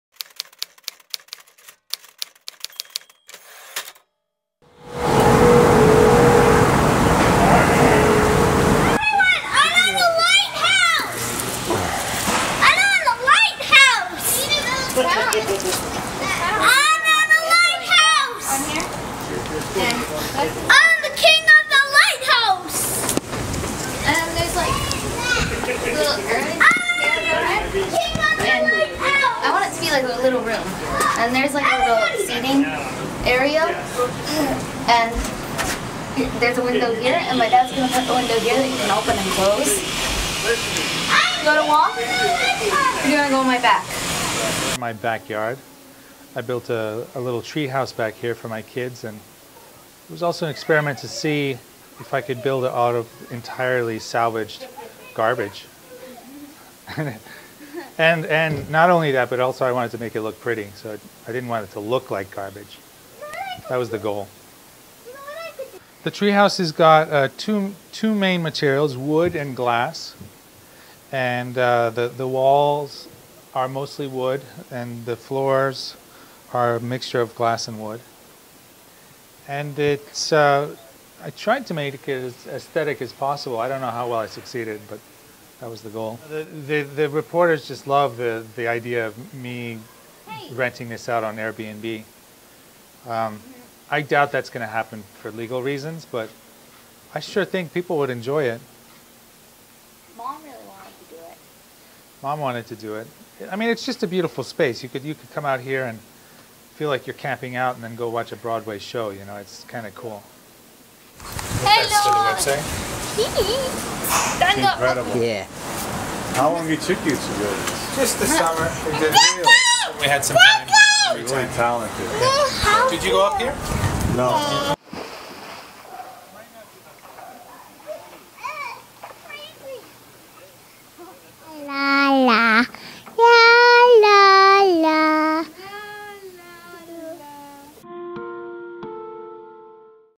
Brooklyn man builds giant treehouse in yard
Carroll Gardens resident Gennaro Brooks-Church built a treehouse in his yard for his kids, and considers it for a BNB. By Todd Maisel 11-11-14